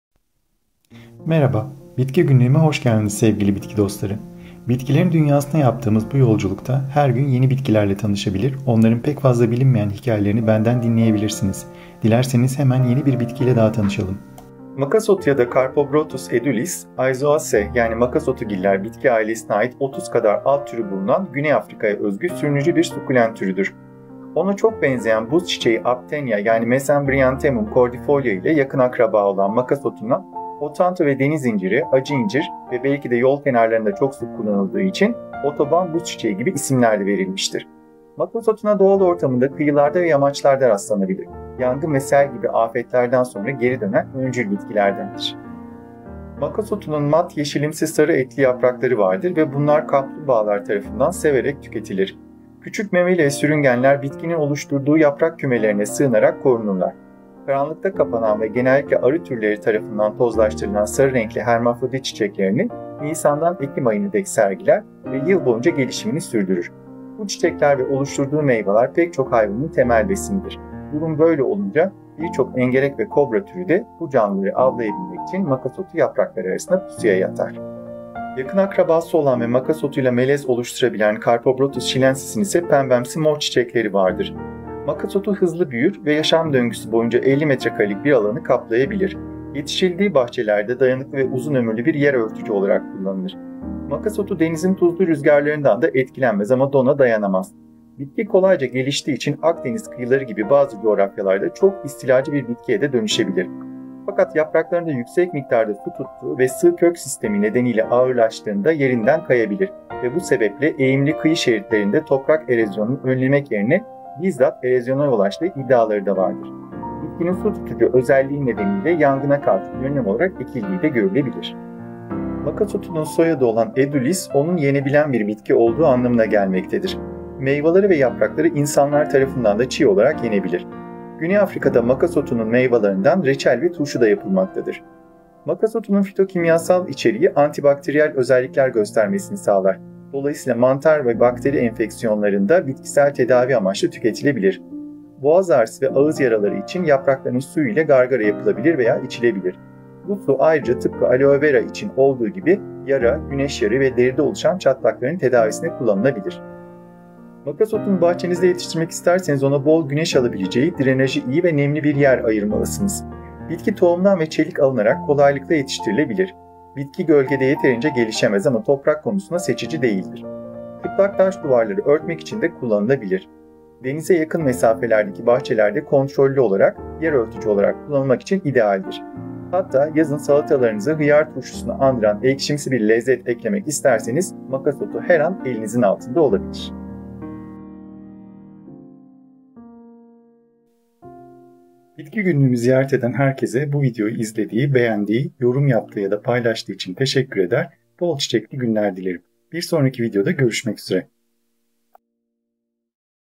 Makasotu - Carpobrotus edulis
"Makasotu ya da Carpobrotus edulis, Aizoaceae yani makasotugiller bitki ailesine ait 30 kadar alt türü bulunan Güney Afrika’ya özgü sürünücü bir sukkulent türüdür. Ona çok benzeyen buz çiçeği Aptenya yani Mesembryanthemum cordifolia ile yakın akraba olan makasotuna, Hotanto ve deniz inciri, acı incir ve belki de yol kenarlarında çok sık kullanıldığı için otoban buz çiçeği gibi isimler de verilmiştir. Makasotuna doğal ortamında kıyılarda ve yamaçlarda rastlanabilir. Yangın ve sel gibi afetlerden sonra geri dönen öncül bitkilerdendir."
Okan Akkoyunlu
Videodaki görseller: Yandex